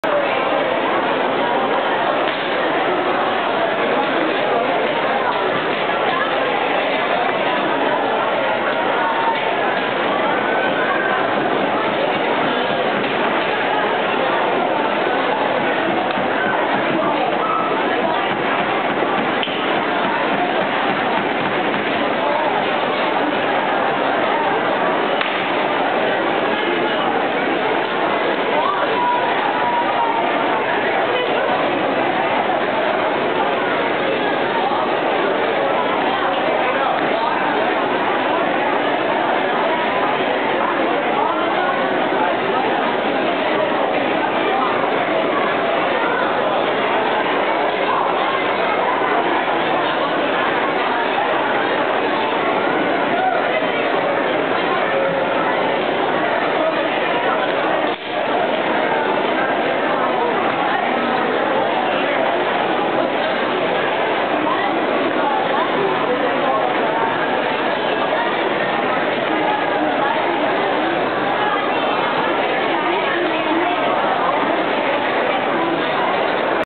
Rube Goldberg Machine Contest 2009 at the Fay School in Southborough, MA Saturday April 4th - MOV2
Looking down to the gymnasium floor where the Teams were assembling their machines.